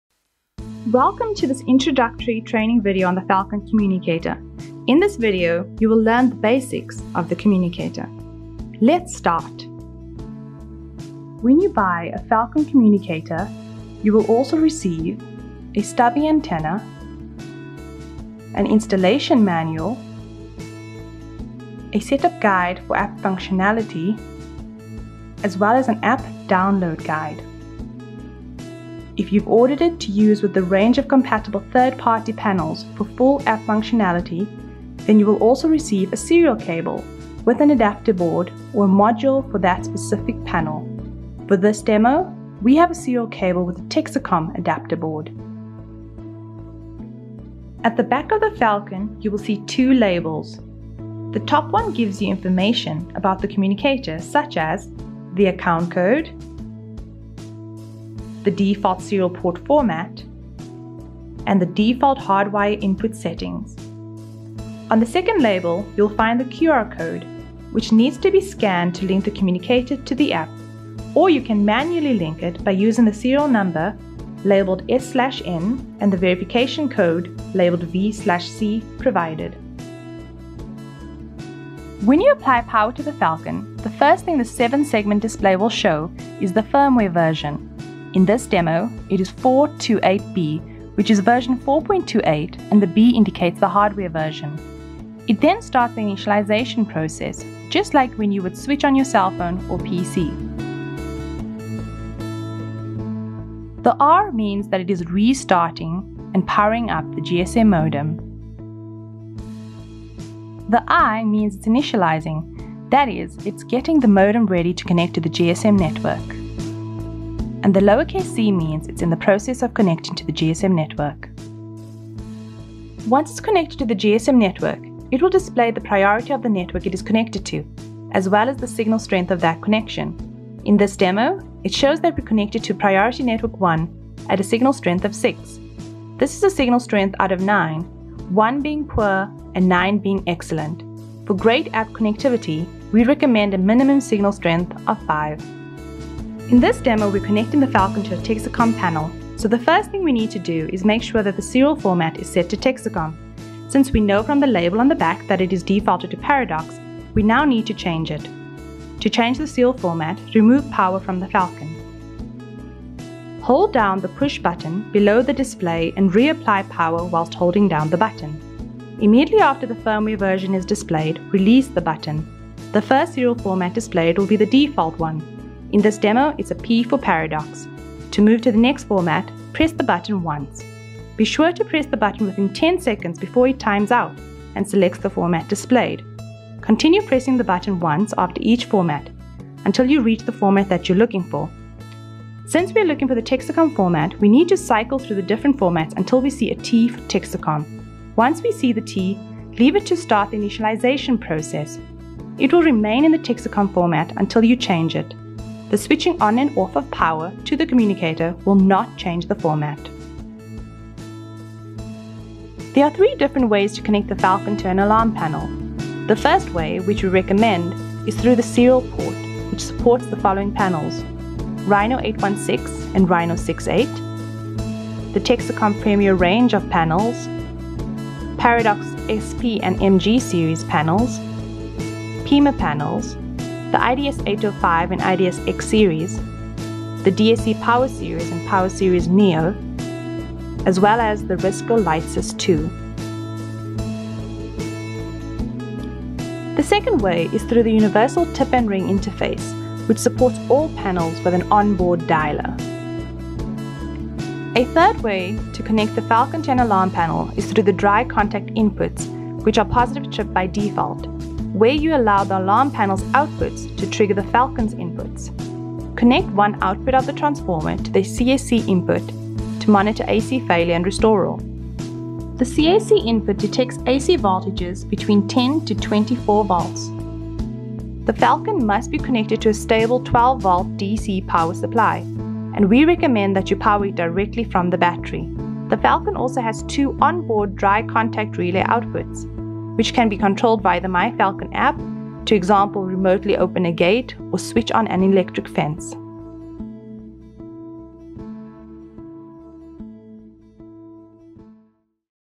Learn how to work with the Falcon communicator
A short introduction explaining the basics of the Falcon communicator.